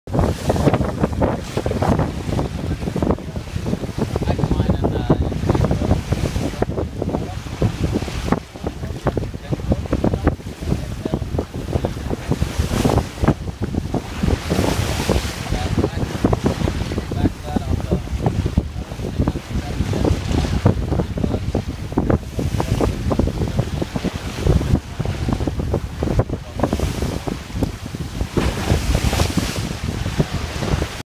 X-Yachts XC 35
This video shows the newly released X-Yachts XC-35 doing her thing on the Chesapeake Bay. Wind speed was 15 kts true and boat speed was 6.5 kts up wind! Notice how the slender bow section of the boat slices through the water rather than pounds. We cracked off to a close reach and saw our boat speed hit 8.9 kts. This boat is a remarkable sailor and has to be sailed to be believed. She is laying in Annapolis and be test sailed by appointment.